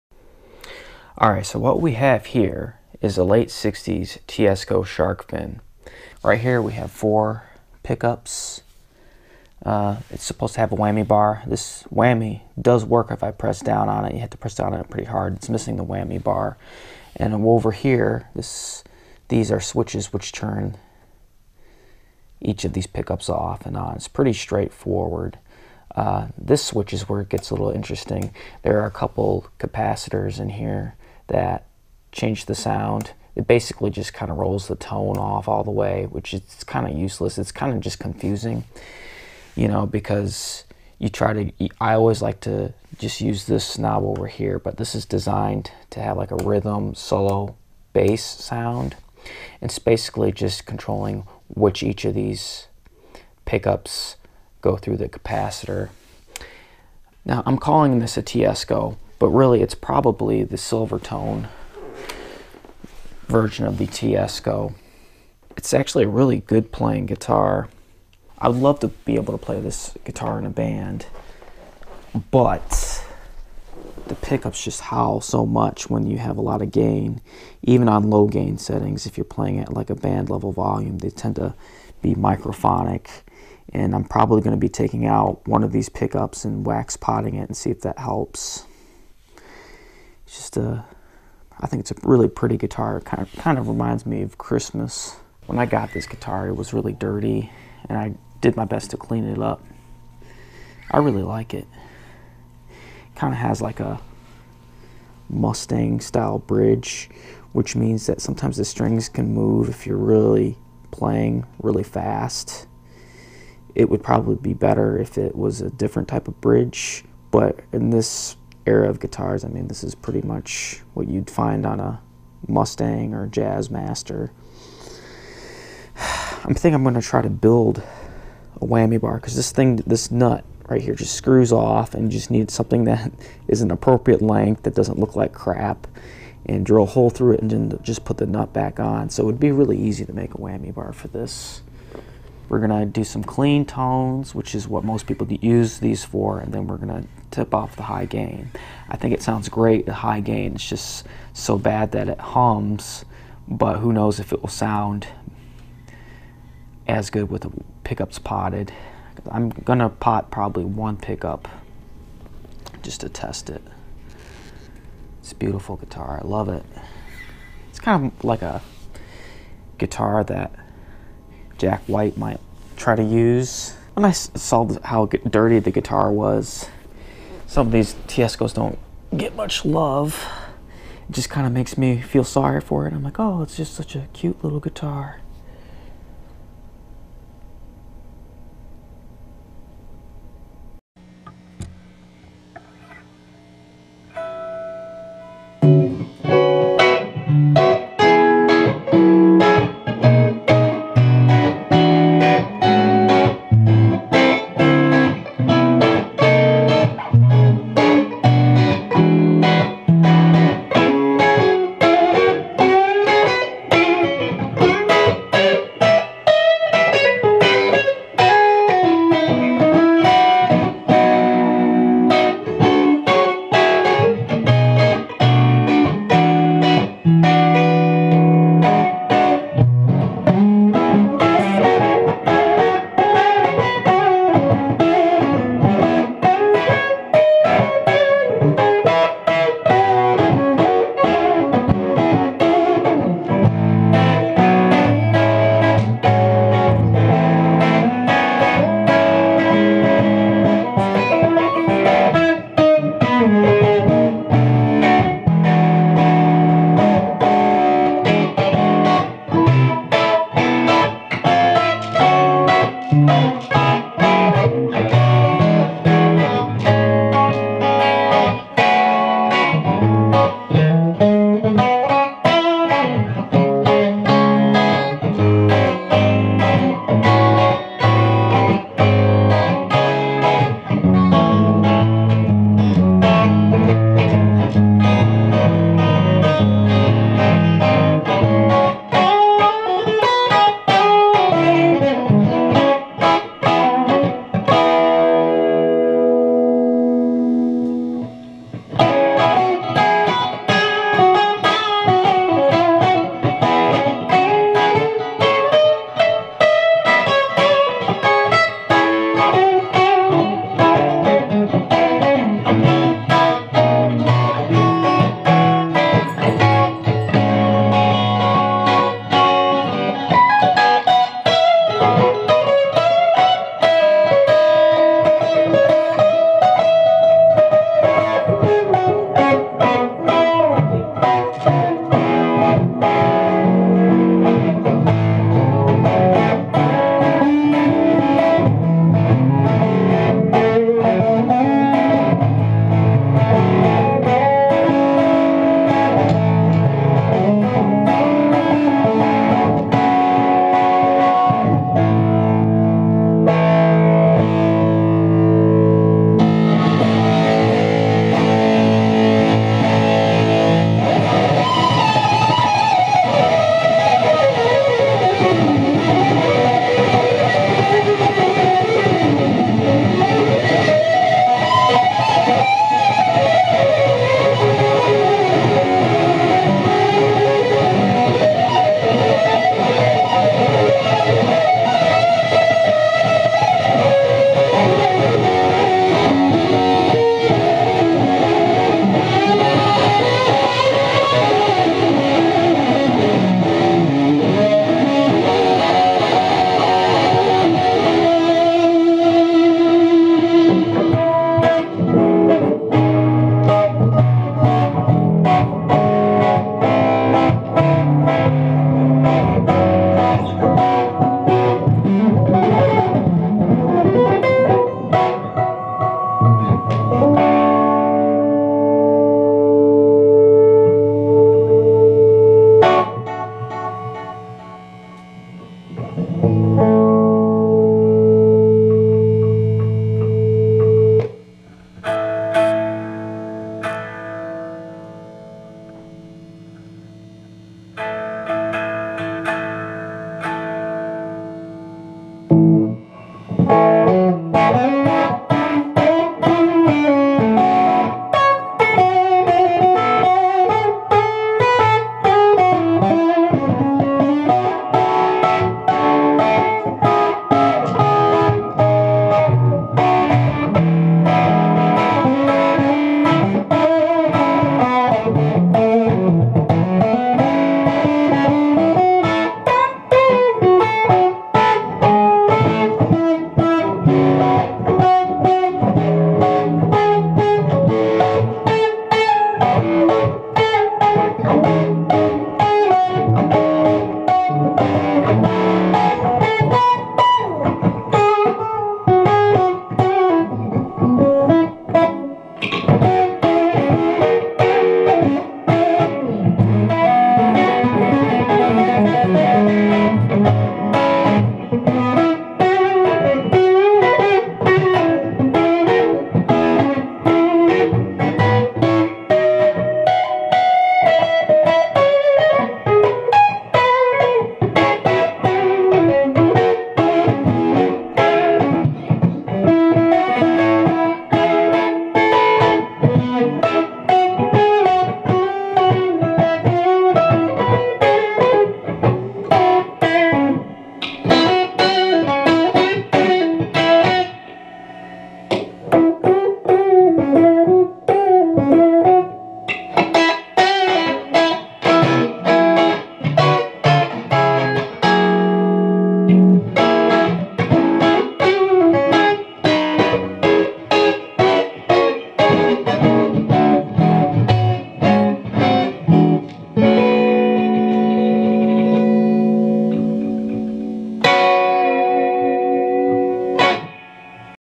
TEISCO SHARKFIN-Demo, impressions, and Opinions
Have you ever heard of a Teisco Sharkfin? They are definitely an unusual sort of guitar from the 1960's that many youngsters back then got for Christmas. They retailed for about half of the cost of their Fender counter parts. Many garage rock and indie musicians may find the look of these appealing. What do I think of it? Watch and find out! You will hear me put this thing through its paces in the second half of the video.